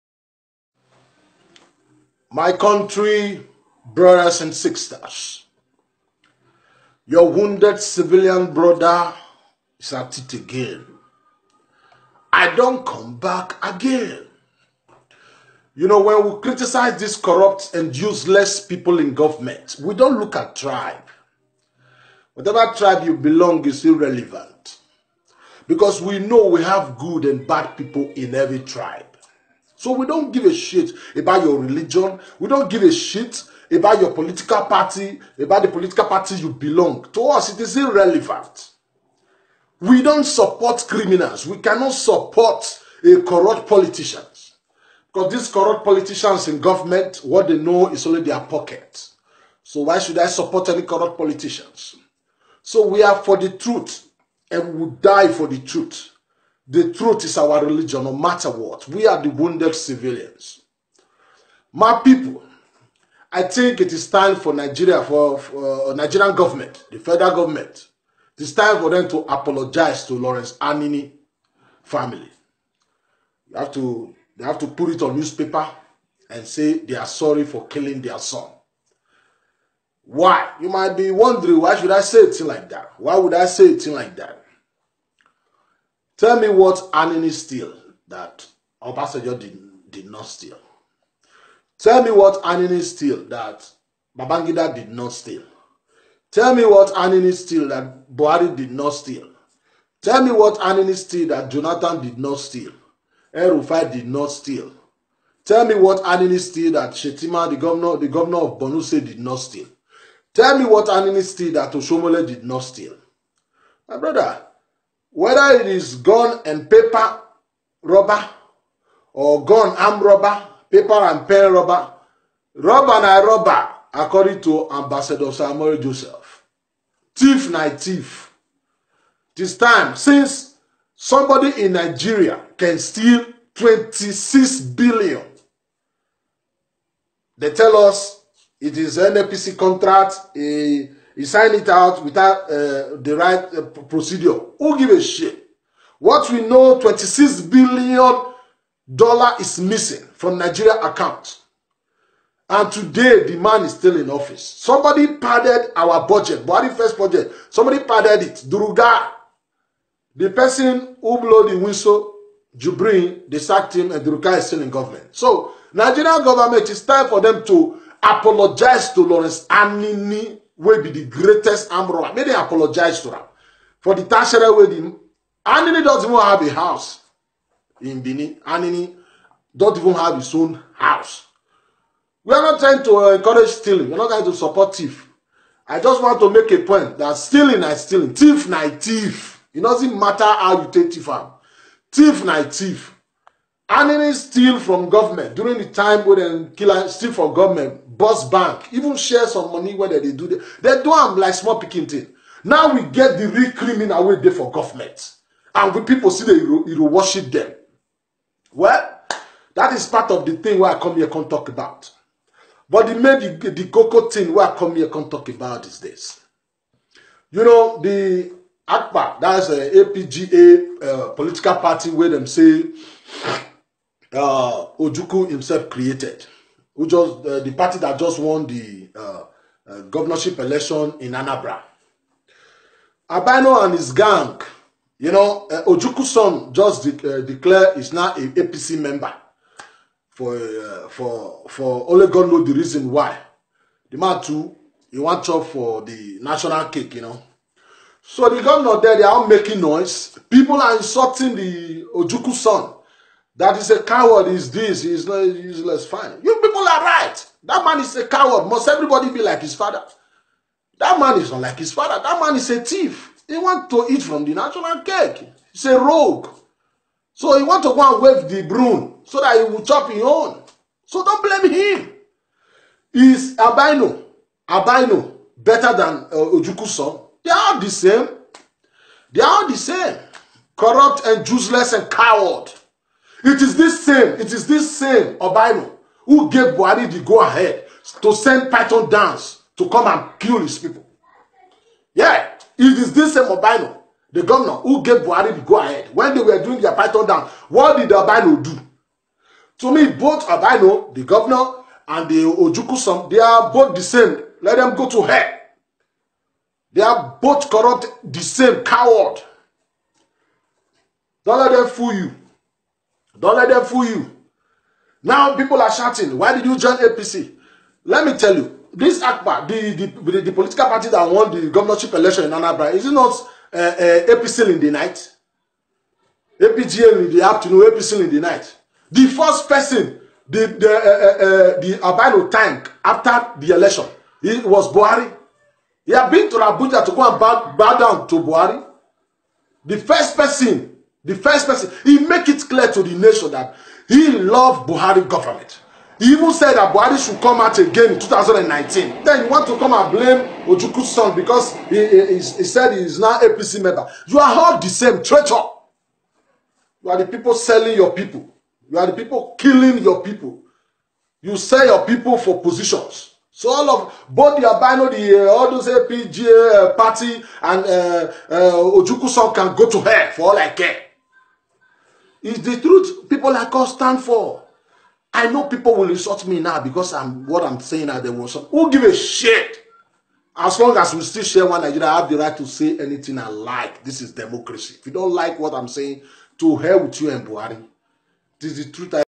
WHAT 'RE THE DIFFERENCES BETWEEN STEALING WITH A BIRO OR WITH A GUN?
NIGERIAN GOVERNMENT SHOULD  APOLOGIZE TO LAWRENCE ANINI'S FAMILY, BECAUSE HE WAS WRONGFULLY KILLED, ACCORDING TO THIS MAN IS HE RIGHT?